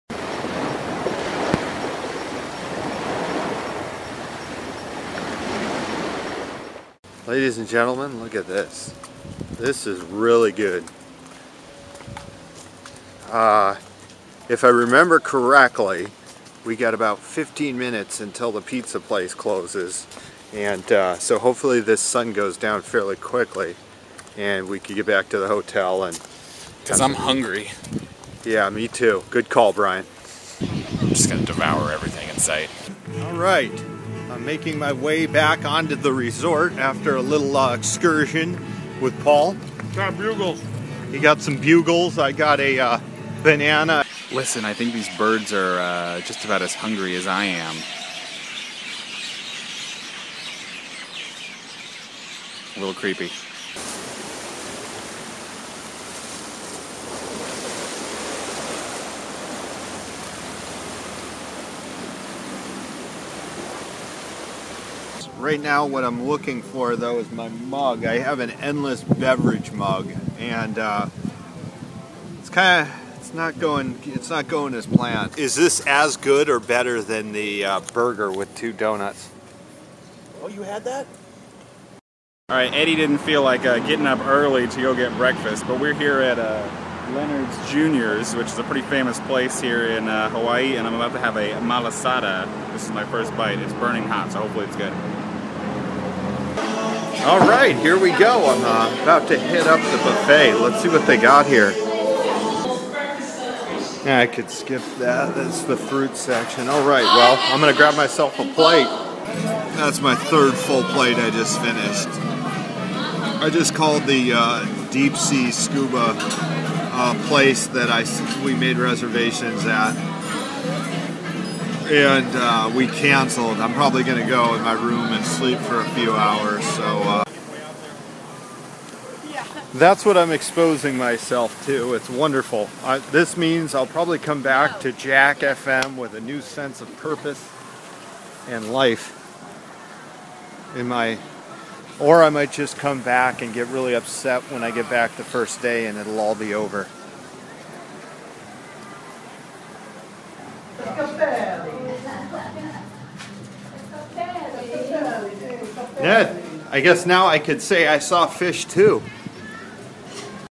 JACK TV - Surplus Session #1.
Eddie the Web Guy and Brian from Promotions, of JACK-FM, go to Hawaii and all they can thinking about is food and eating.